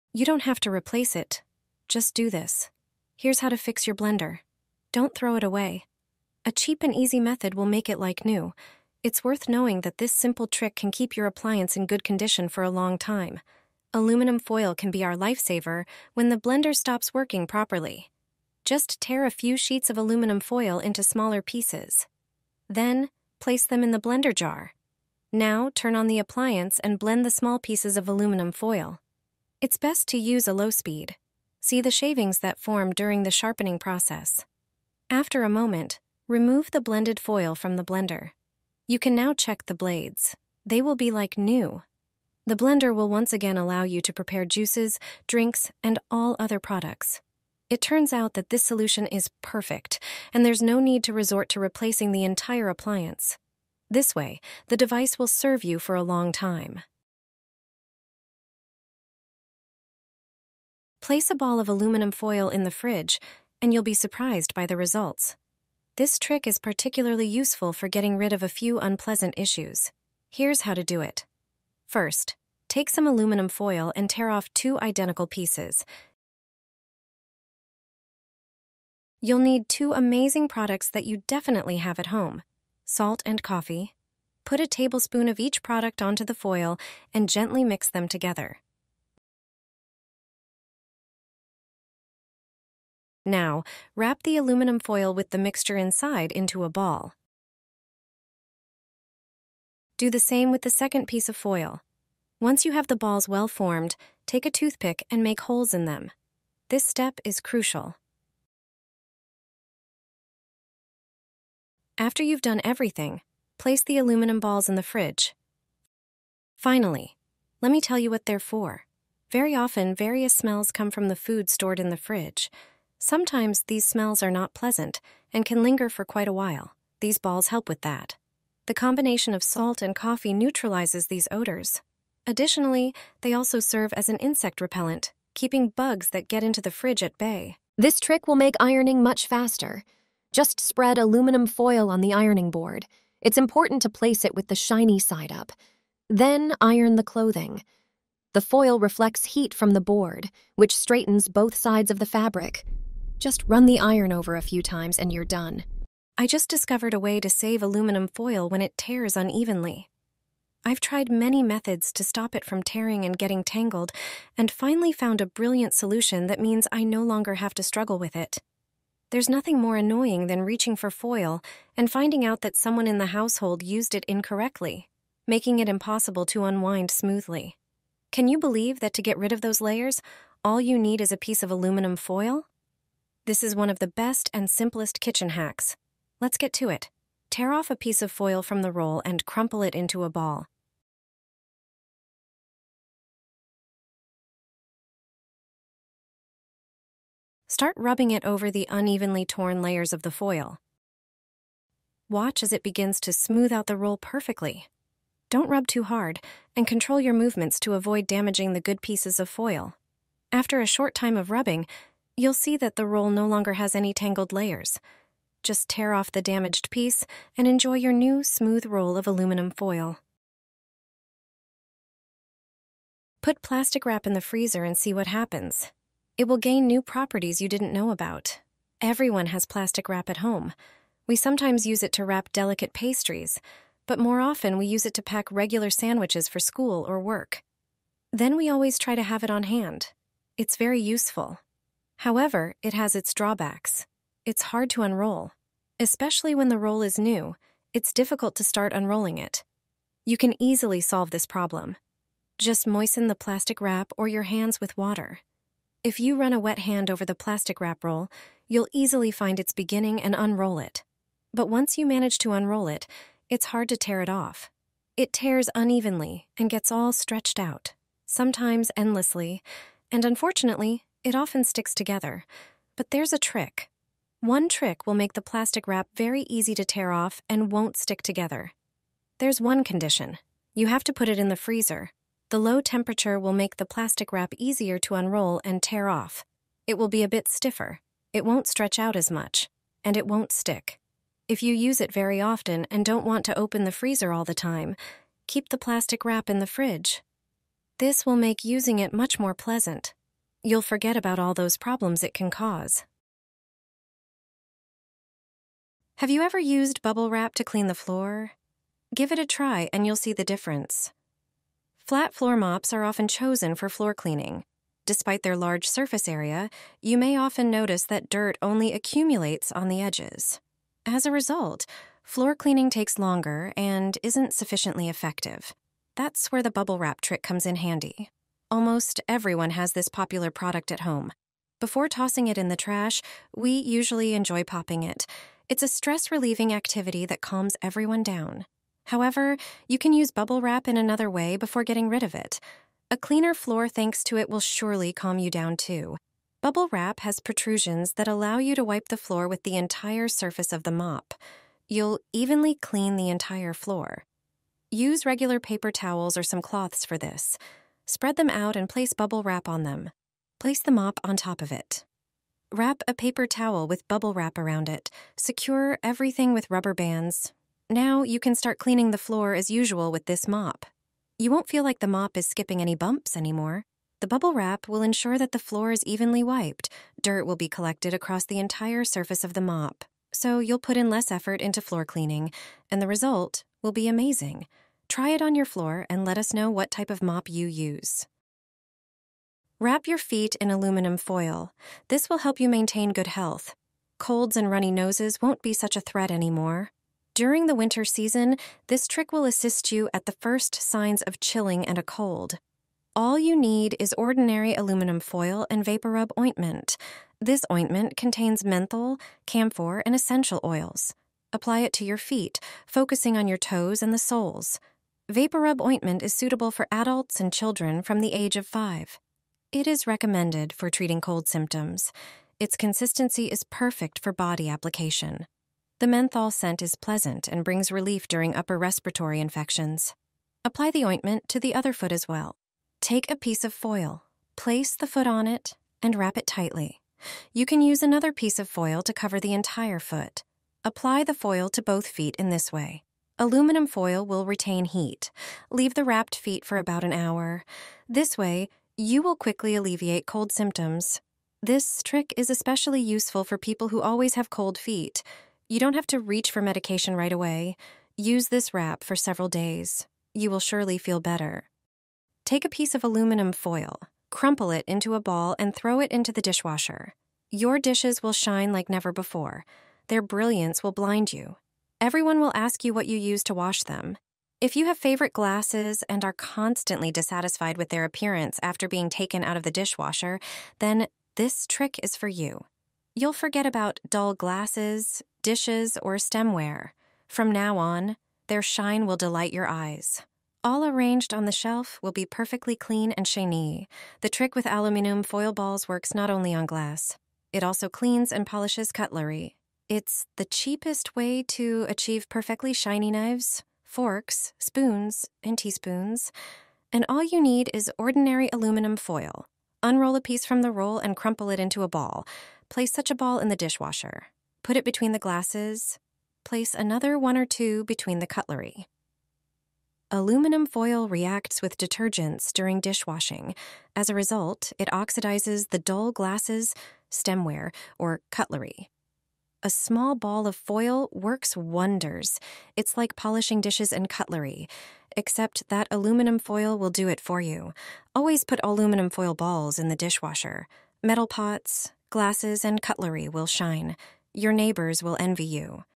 Put pieces of THAT in the blender and blades will be as good as new
How to sharpen a blender?